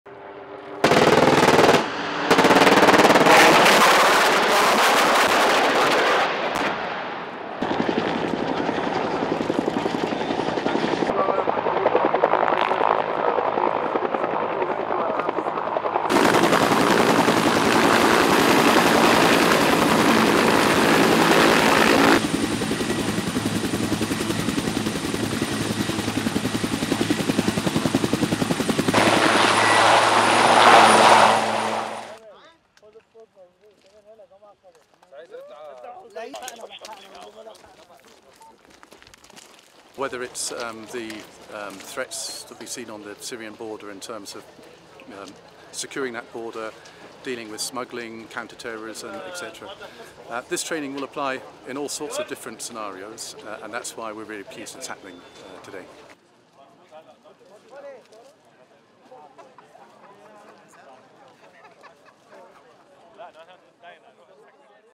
British and Lebanese military conclude 10-day combined exercise
A joint UK-Lebanon combined exercise, Pegasus-Cedar, ran for 10 days in Aqoura, 70km north of Beirut.